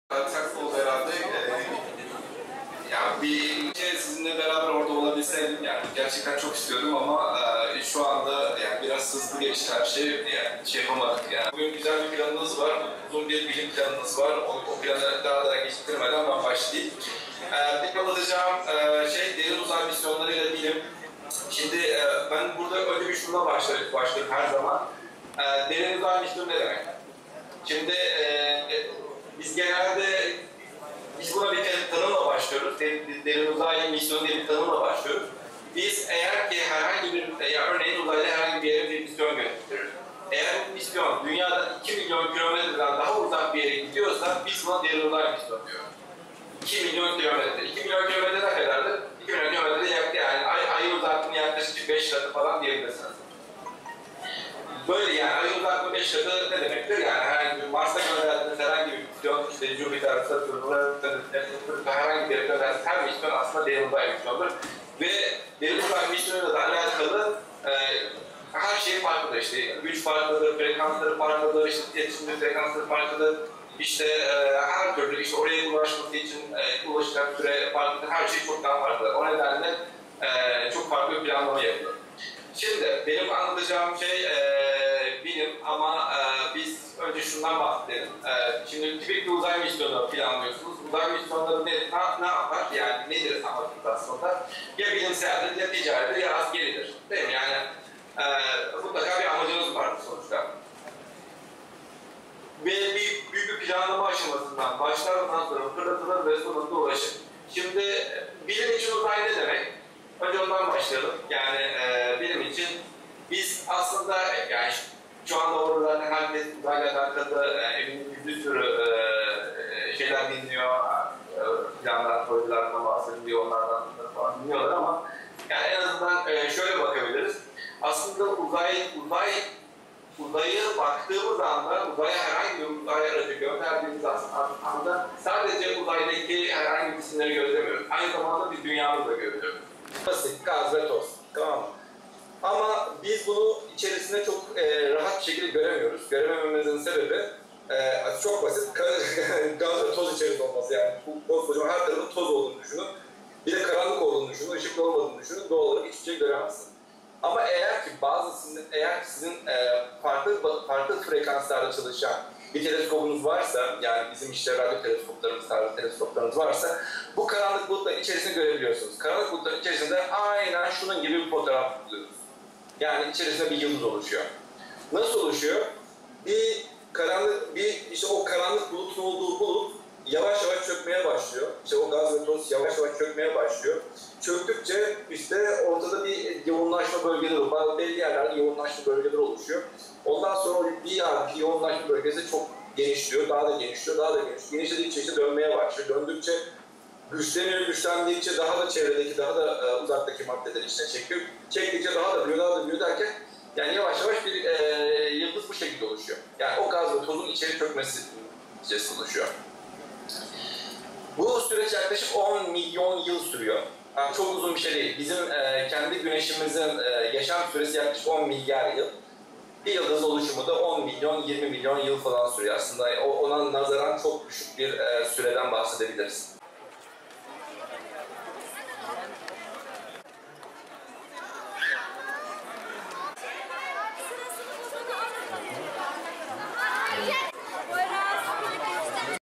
Teknouzay bilim şenliğinde NASA’ya canlı bağlantı yapıldı…
"TÜBİTAK 4007 Bilim Şenlikleri Programı" kapsamında desteklenen 'İskenderun Teknouzay Bilim Şenliği" İste de hayata geçti.